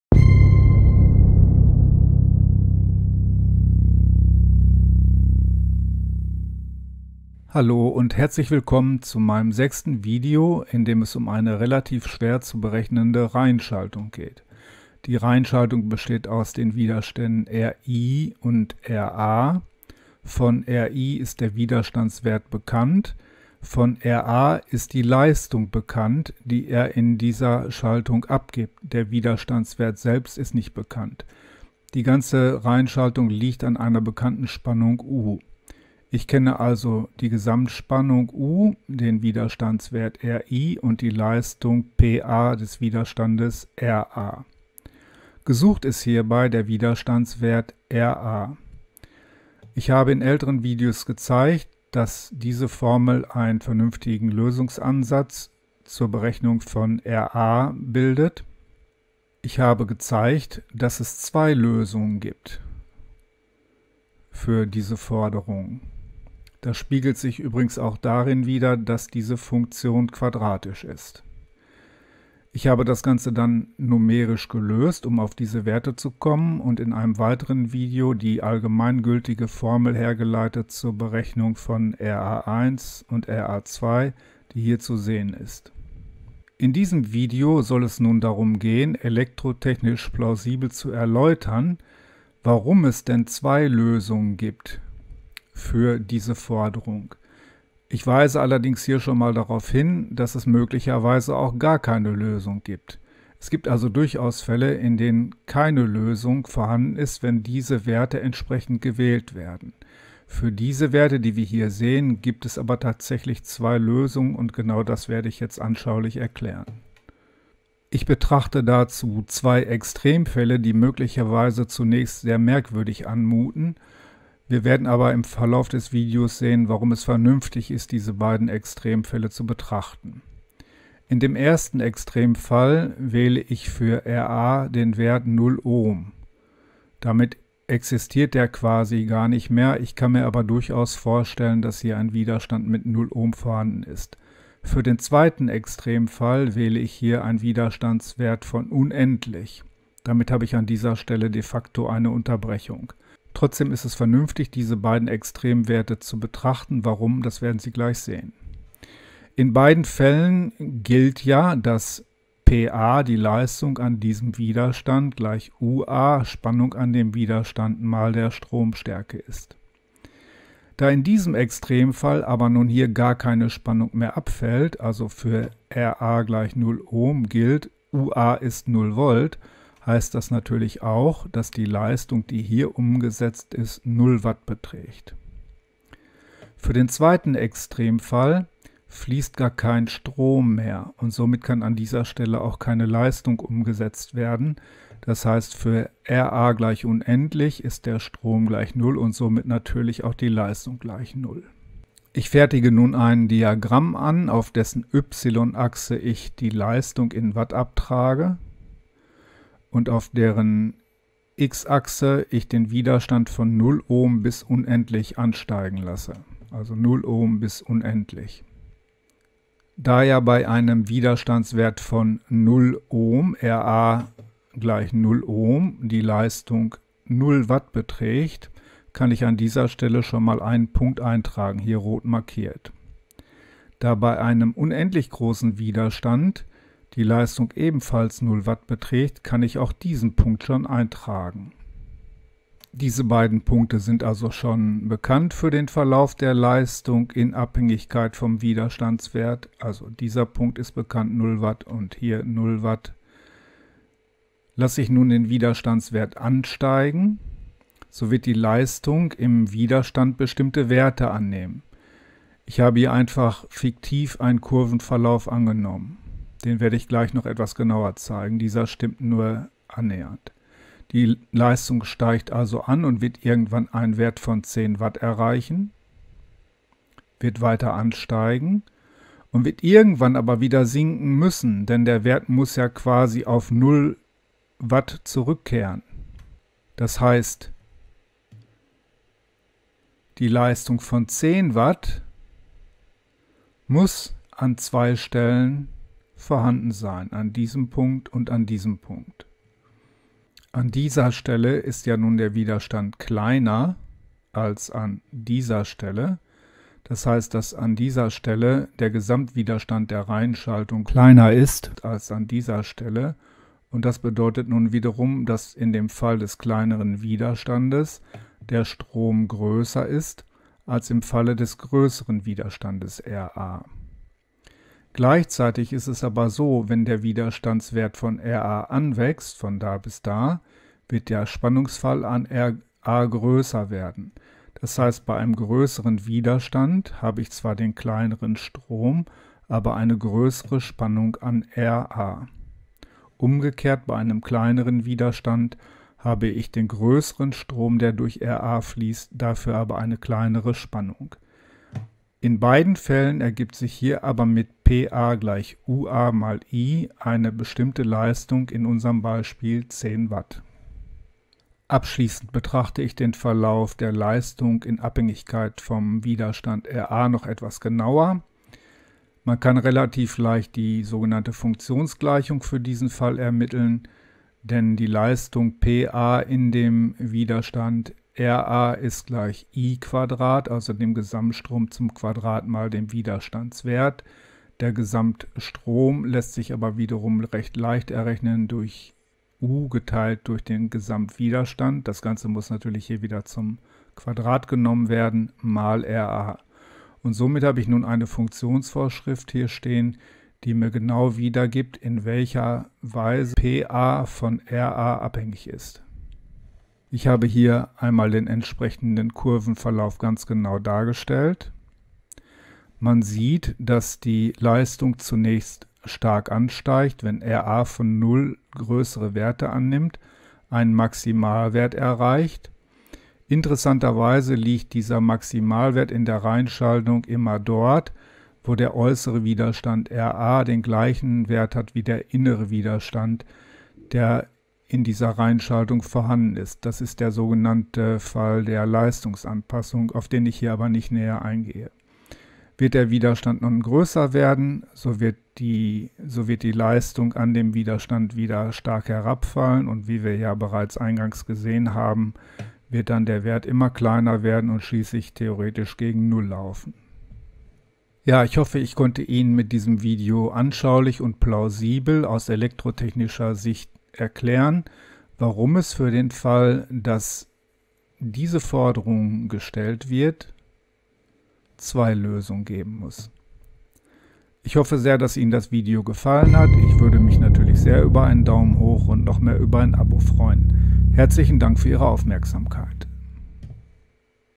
Teil 6: Schwere Aufgabe zur Reihenschaltung / Warum 2 Lösungen ? Elektrotechnik für Fortgeschrittene
Anschauliche Erläuterung für die Existenz von 2 Lösungen für die Aufgabenstellung. Mit einem (Mini-) Ausblick auf die Leistungsanpassung.

Falls Sie mich unterstützen wollen, kaufen Sie über den folgende Link bei Amazon.
(Ohne Mehrkosten, egal welches Produkt.)